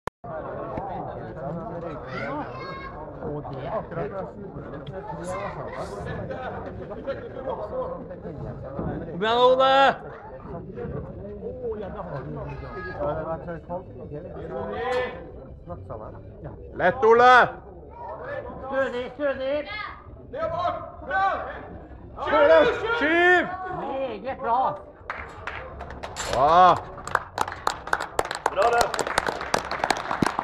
Ole Morten Kristiansen RM 2010 Squat 2 200kg fail.mp4
Ole M squats 200 kg easy but the judges claims its not deep enough.